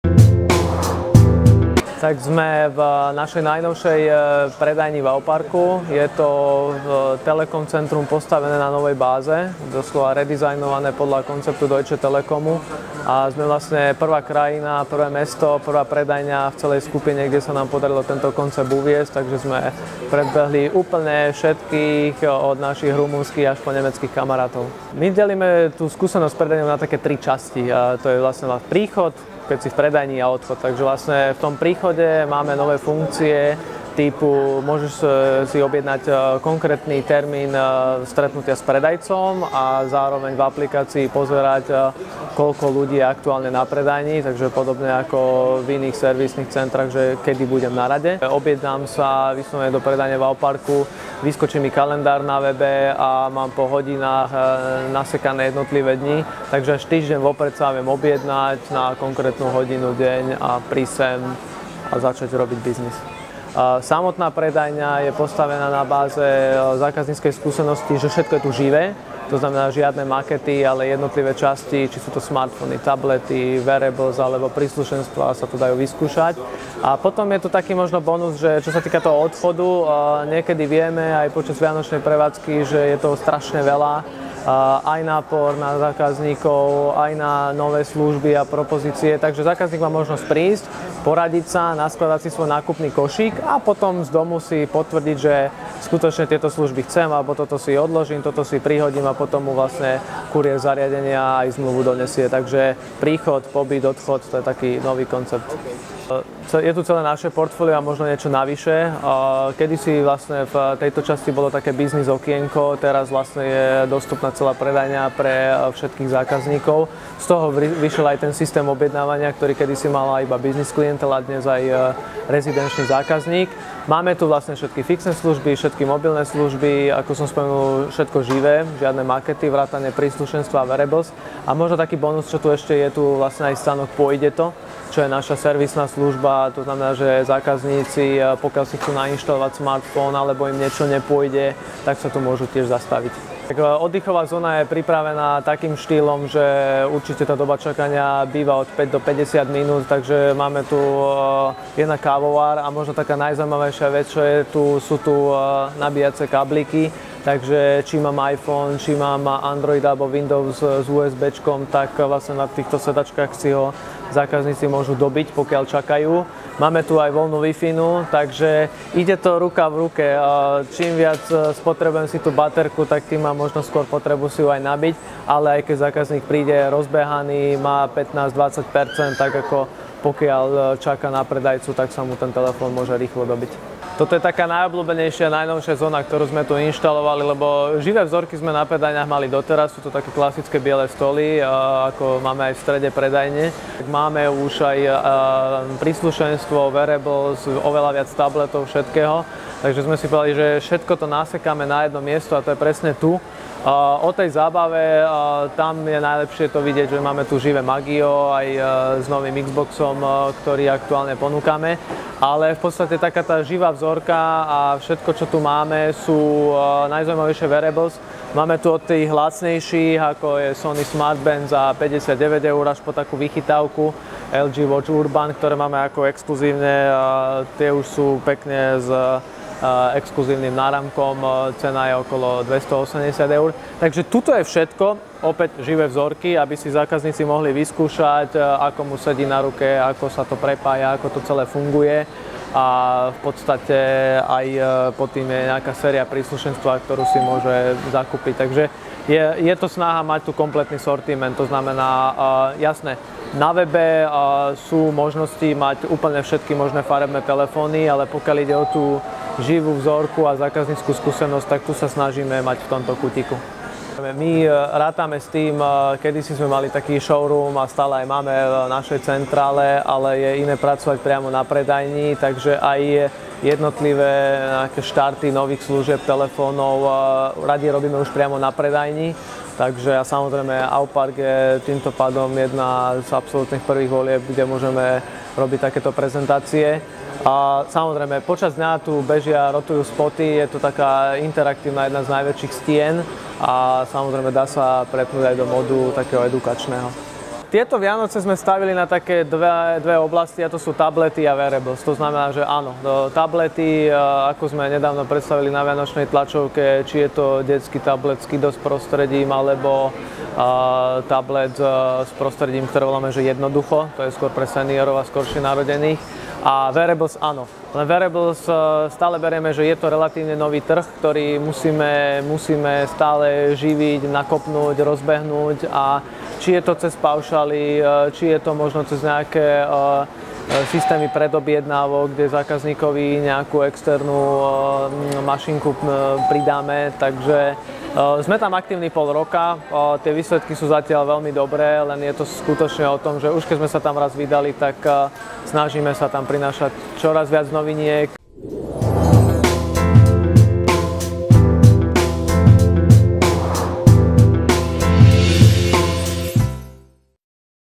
Nová predajňa Telekomu v Auparku
Pozrite si najmodernejšiu predajňu Telekomu v Auparku. Základom sú živé vzorky, možnosť rezervácie termínu cez aplikáciu, Wi-Fi a možnosť nabíjania.